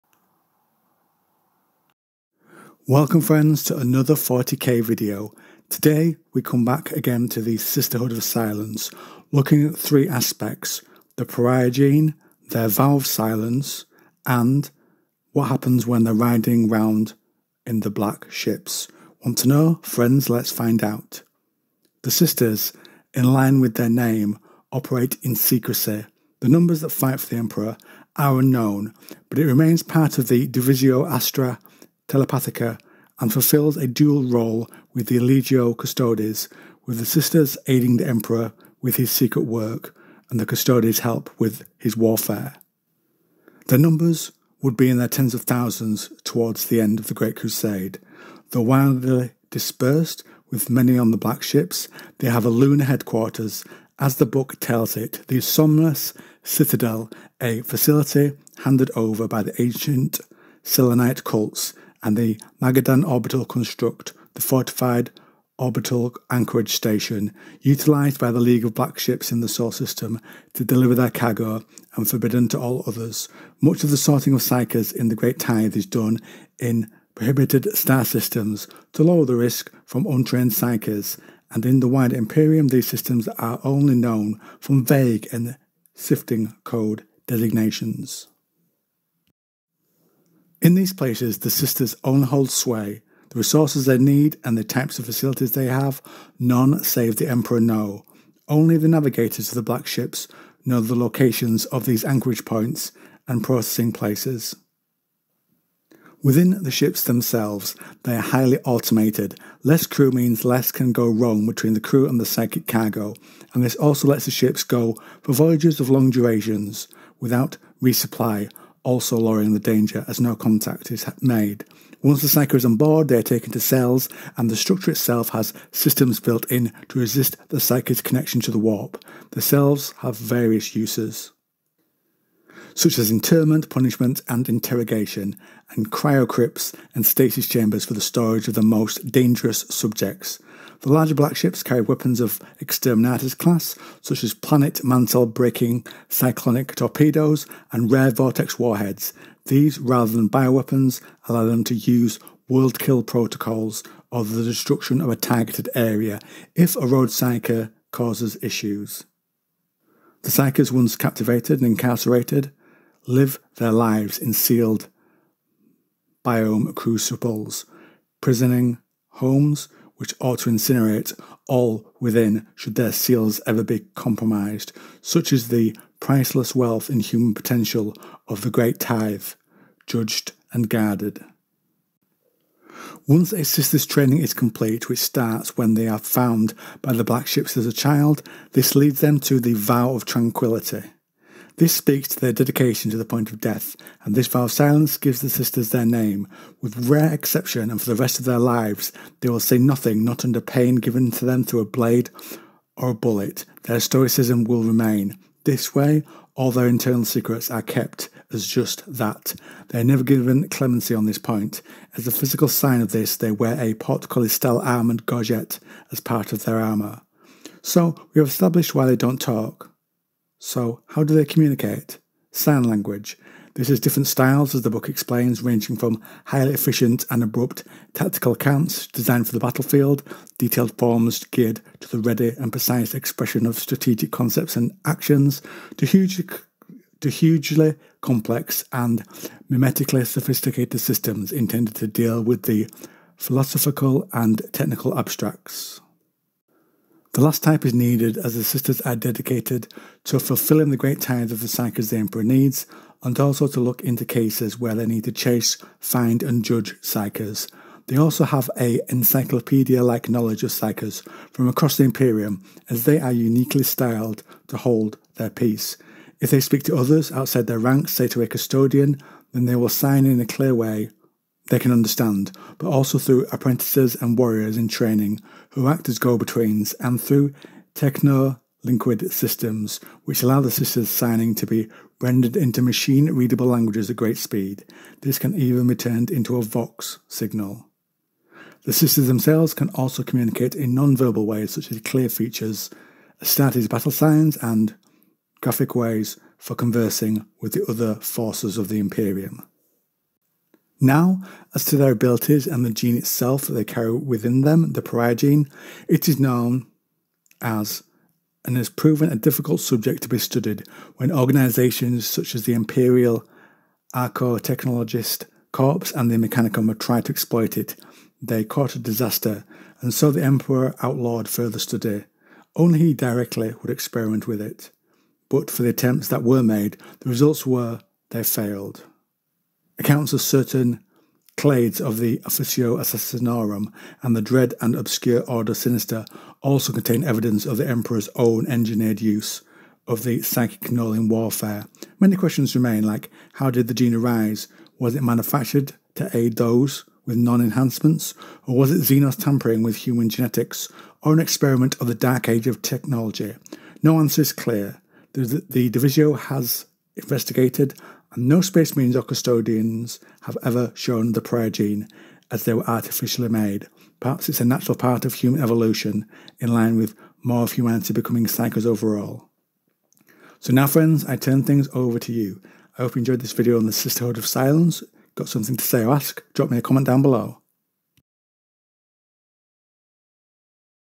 BLACK Ships And The Vow Sisterhood Of Silence 40k Lore Warhammer 40000k Lore
Welcome Friends To Another 40k Lore, we examine three aspects of the sisterhood, the Pariah Gene, The Black Ships and Why the vow of silence? Friends lets find out!